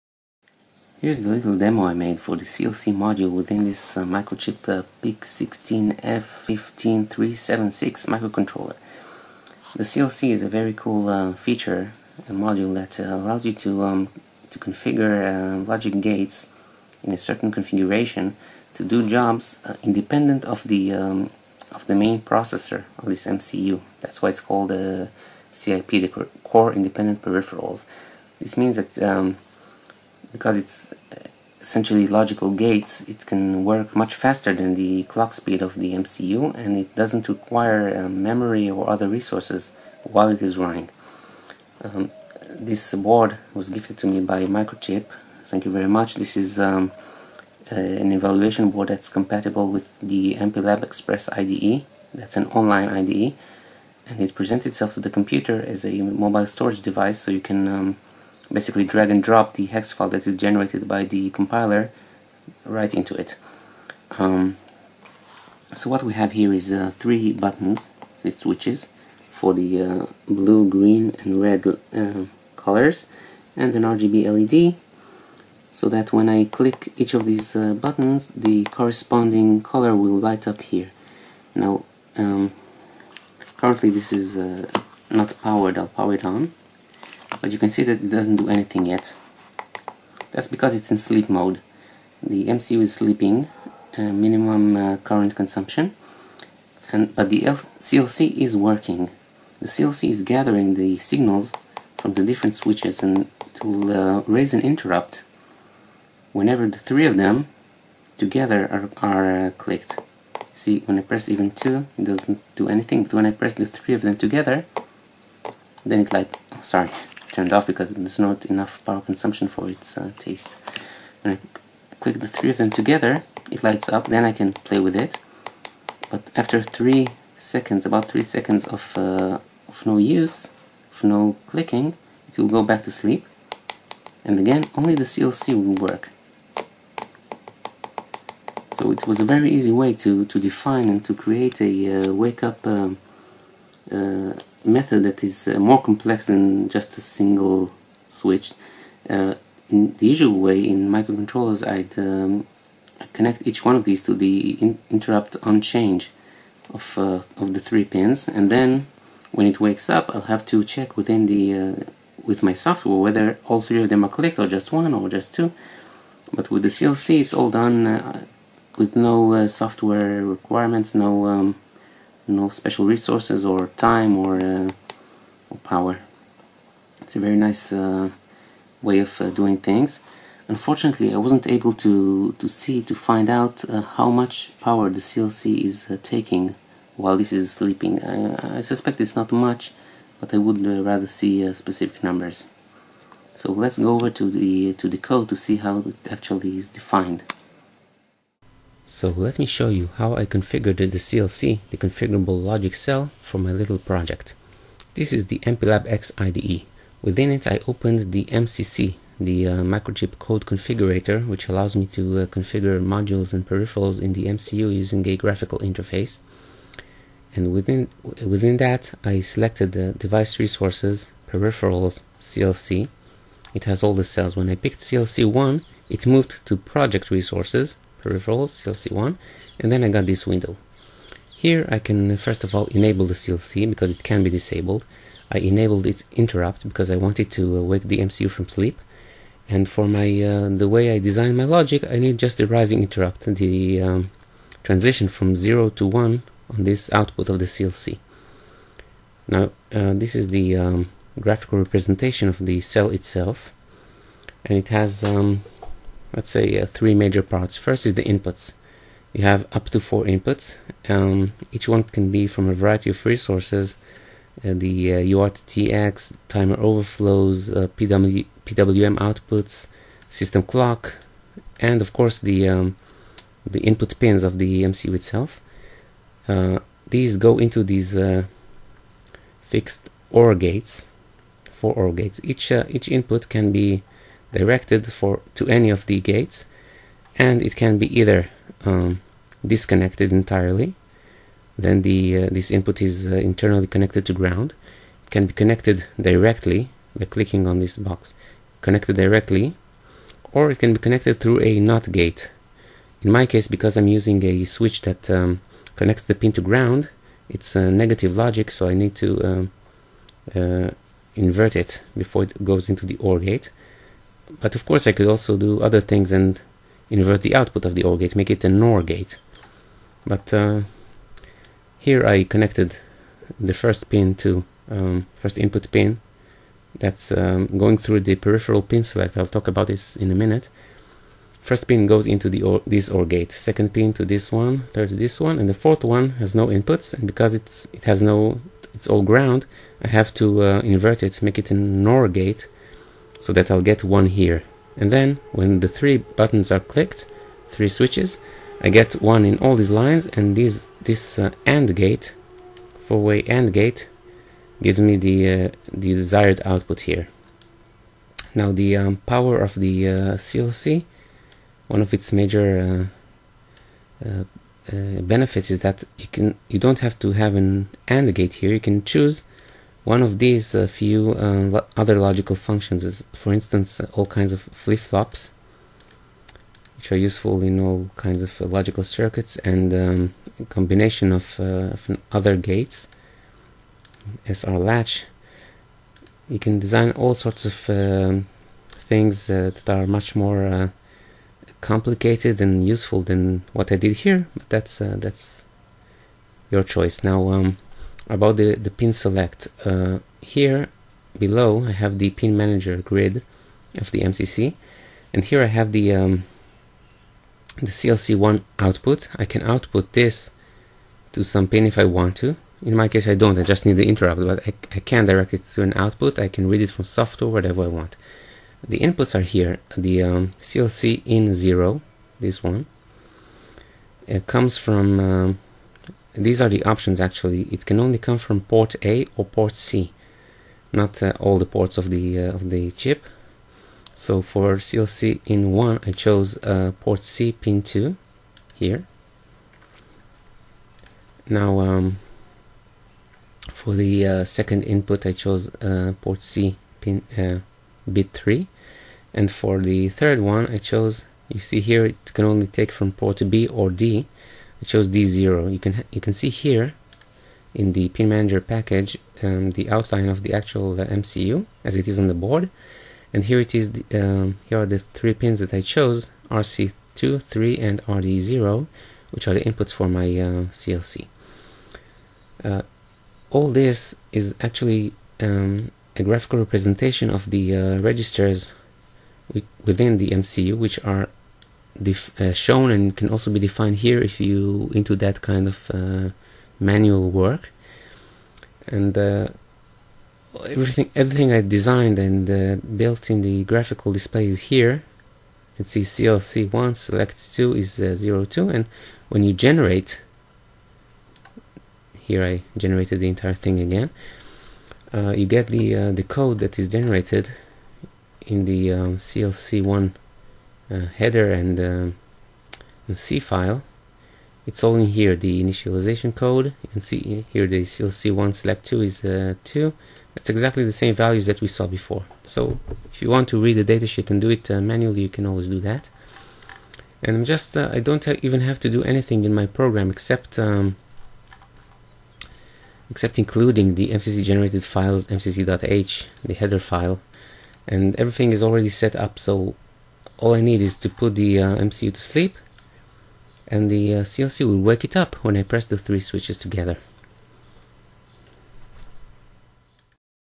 PIC CLC Demo and Configuration
In this little demo project, I used a CLC - Configurable Logic Cell - on a PIC MCU to make a system wake up from sleep only when the three input switches are pressed simultaneously.

The CLC can combine signals from different sources and run simple logic operations on them without occupying the processor or other resources. This can save power, code lines etc. and sometimes allow things that simply couldn't be done otherwise with an 8-bit MCU like this.

For nitpickers: Yes, the code I showed could use some clean-up :-)